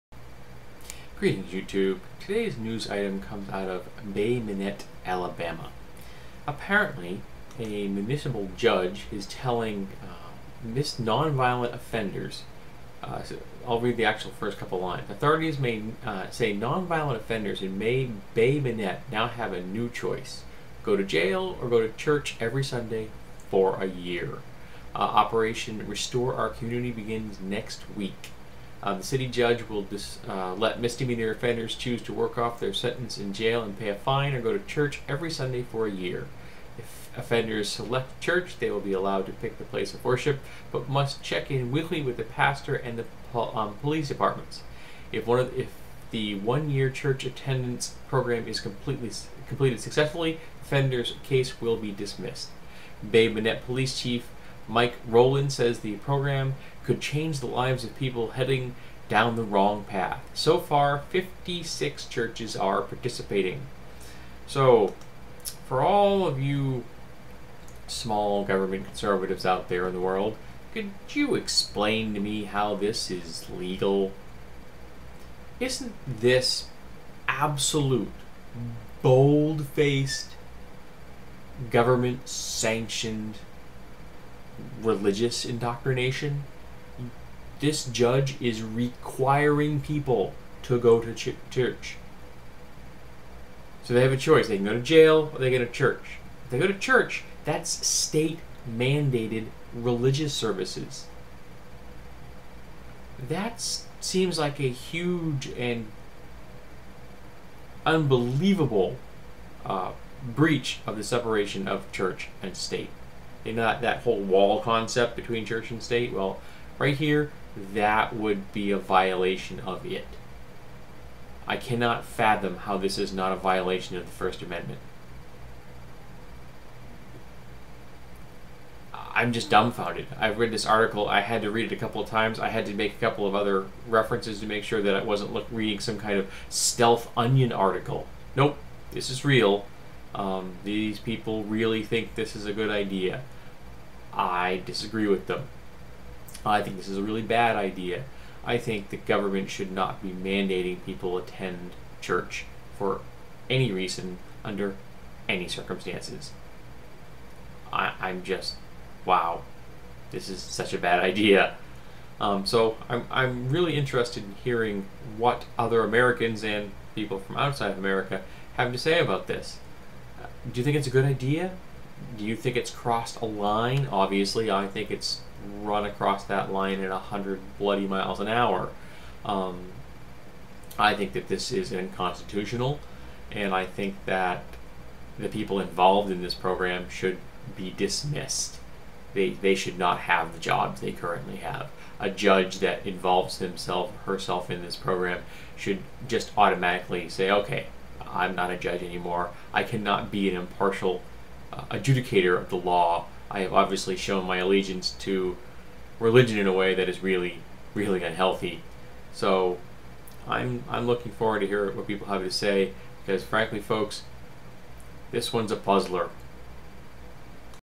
State mandated religious services
Bay Minette, Alabama is giving misdemeanor offenders the chance to attend church rather than serve time. I guess we don't need that pesky 1st amendment any longer. We are a theocracy.